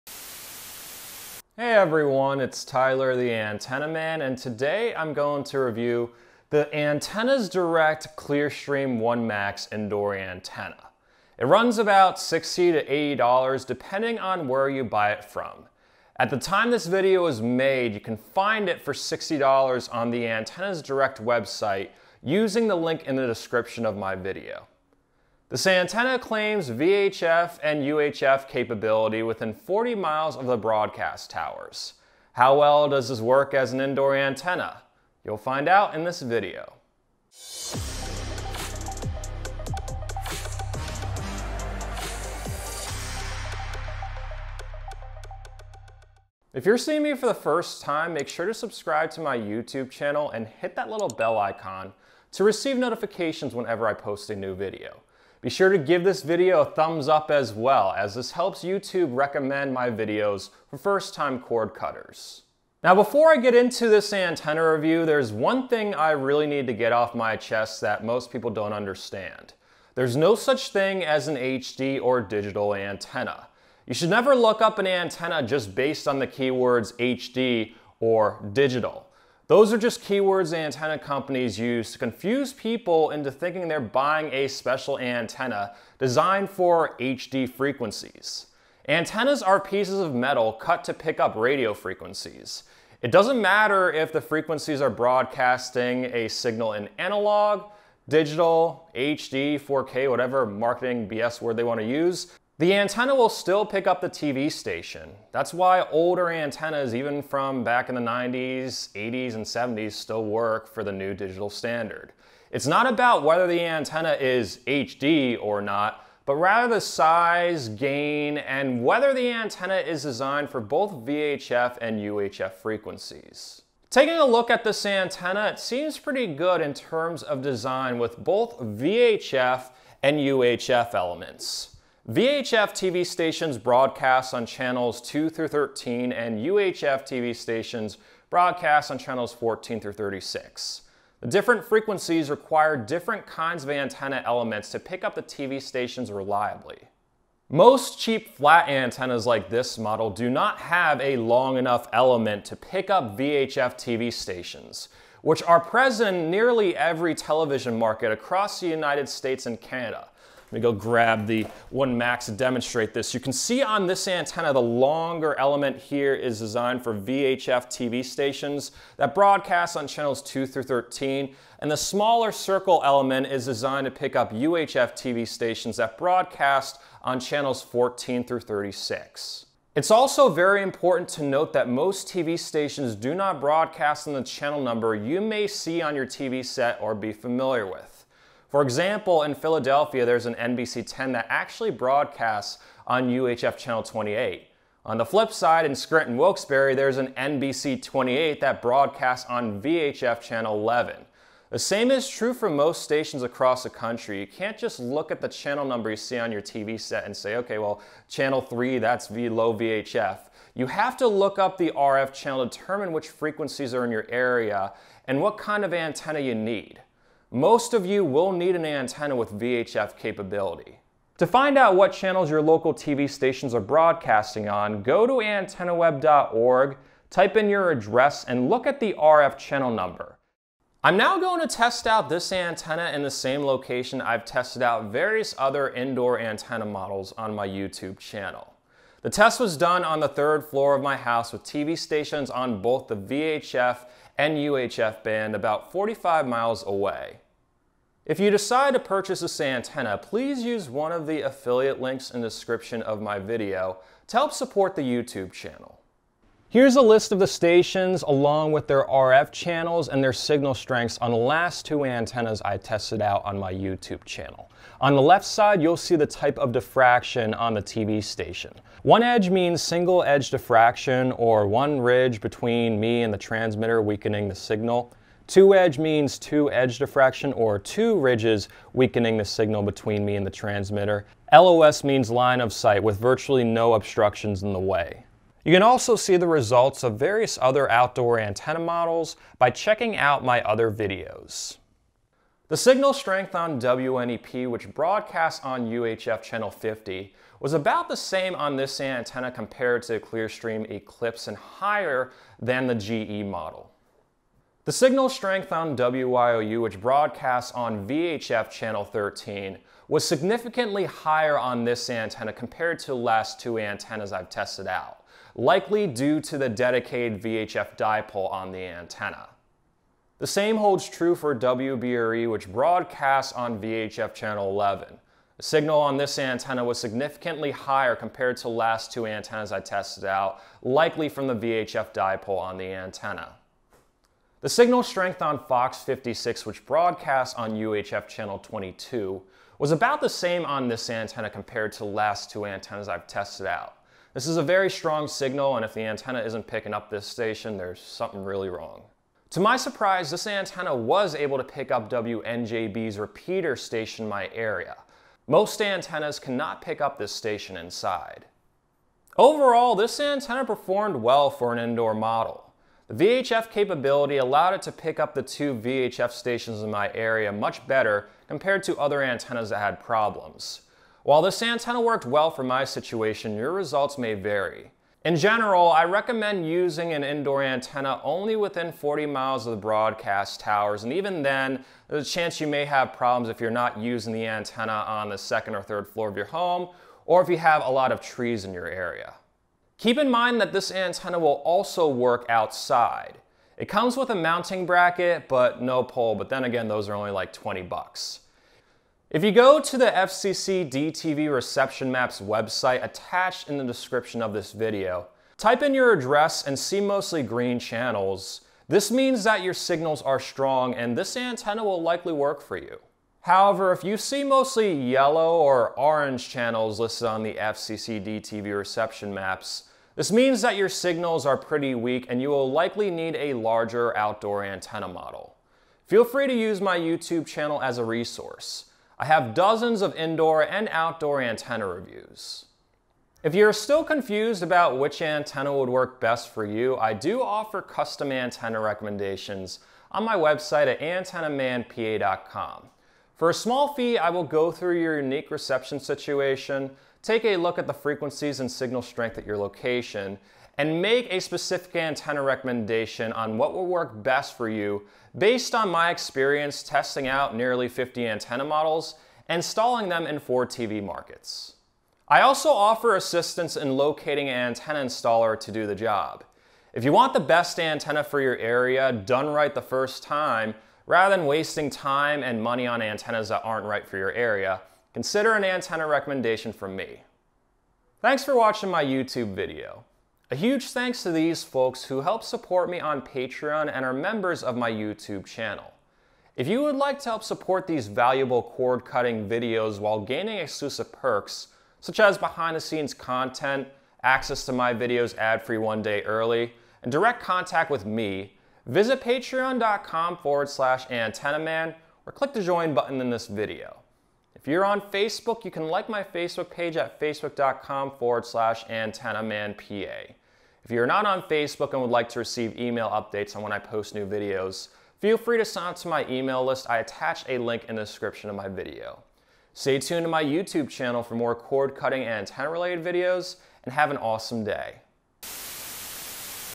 ClearStream 1Max Indoor Outdoor VHF/UHF Antenna Review
In this video I review the Antennas Direct ClearStream 1Max Indoor/Outdoor TV antenna. It runs between $60-$80 online and claims to pick up digital HD TV signals within 40 miles of the broadcast tower. How well does this indoor antenna work and where would I recommend it?

Affliliate Links:

Link to the 1Max on Amazon